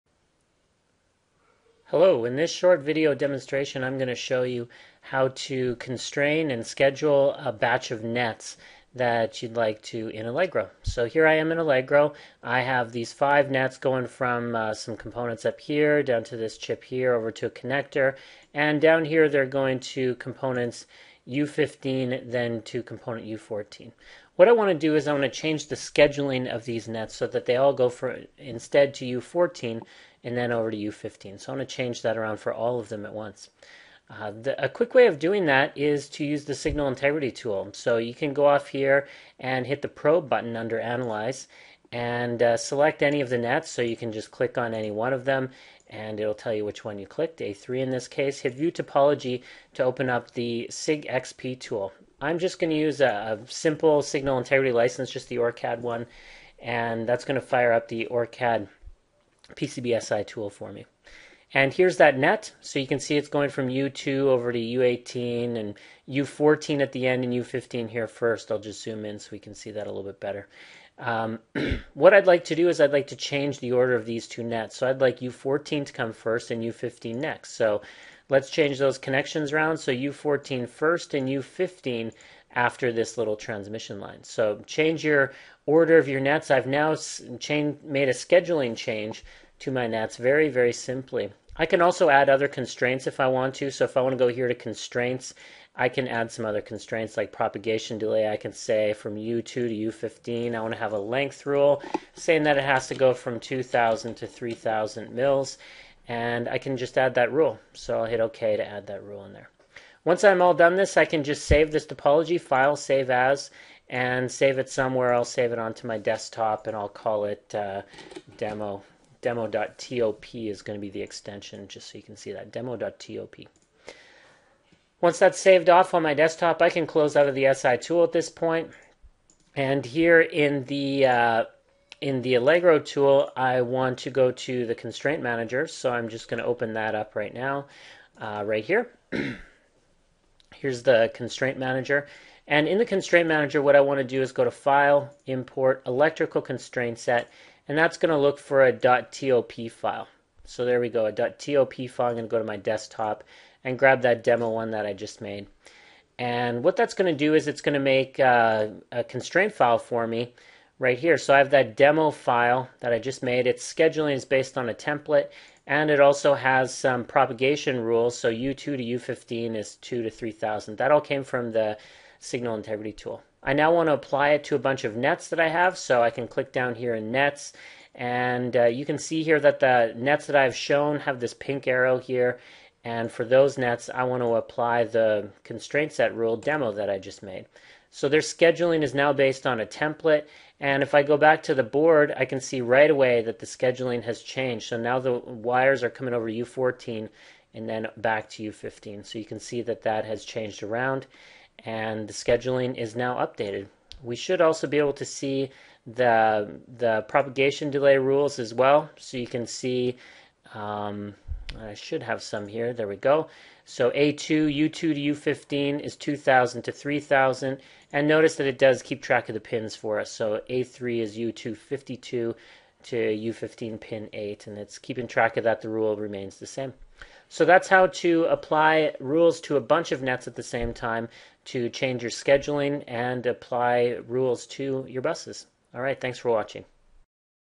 Allegro PCB Create Bus Rules
Use the Signal Integrity tool to create rules and scheduling changes for your group of nets